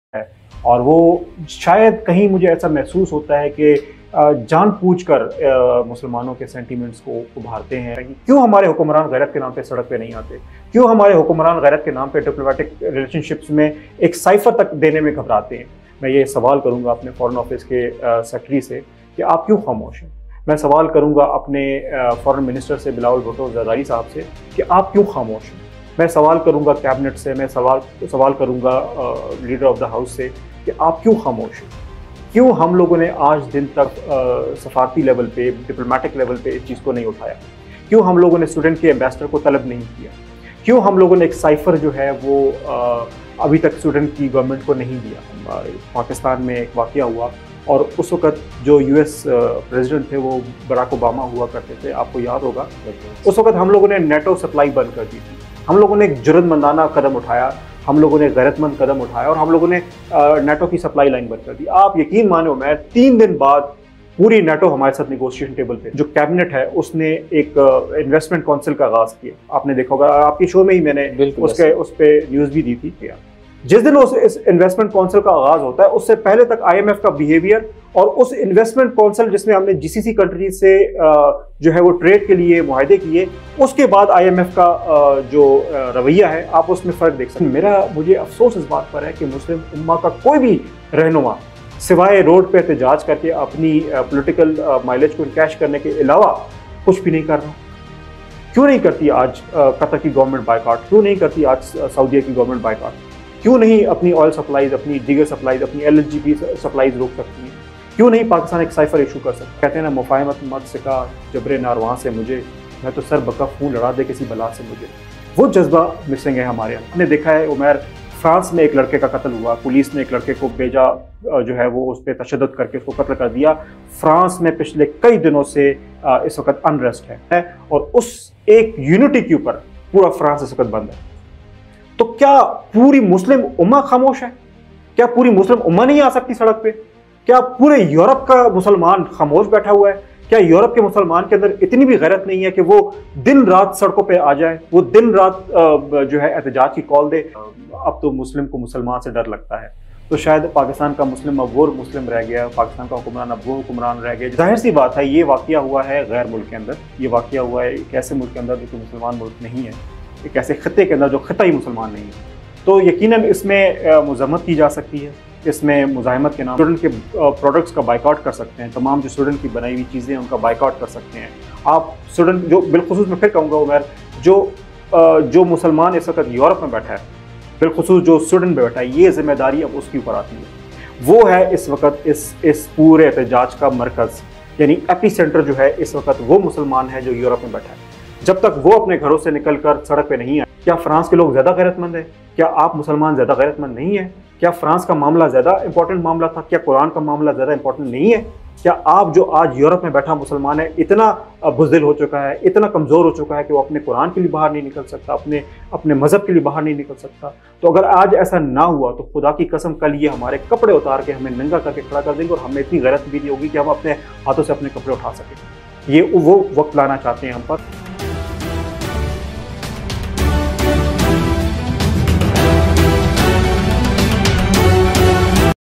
It's testing time for the Muslims of Europe
It's a testing time for the Muslims of Europe, the muslim brotherhood unity is missing & the Muslim Governments are in silence, even the Foreign Office & Cabinet of Pakistan are hesitating to issue a cipher.